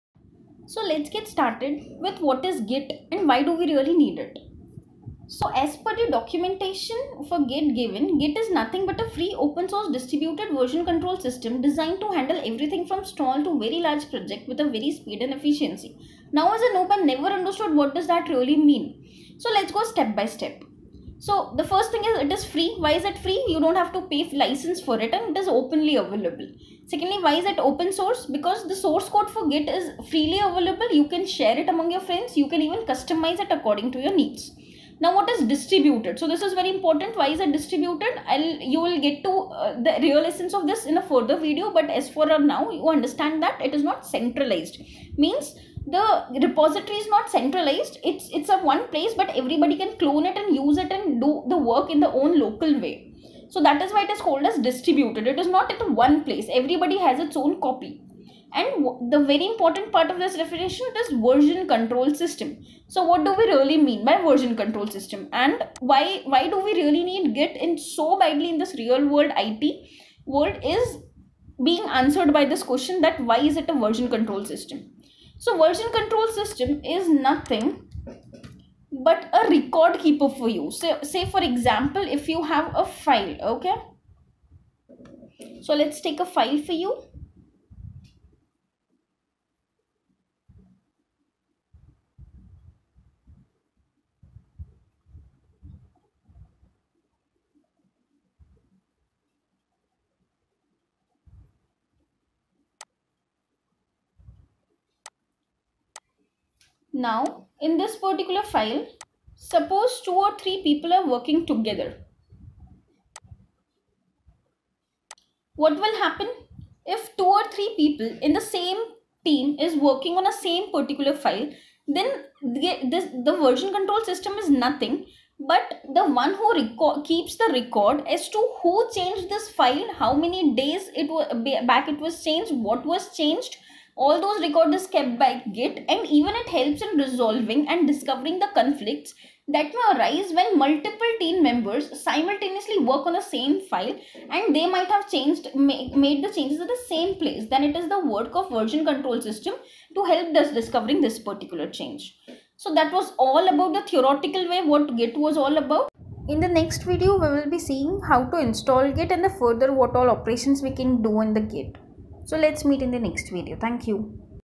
What is git || Why Git || What is Version Control system(VCS) [GIT BASICS]
What is git || Why Git || What is Version Control system(VCS)

Git is a version control system for tracking changes in computer files and coordinating work on those files among multiple people. It is primarily used for source code management in software development, but it can be used to keep track of changes in any set of files. As a distributed revision control system it is aimed at speed, data integrity, and support for distributed, non-linear workflows.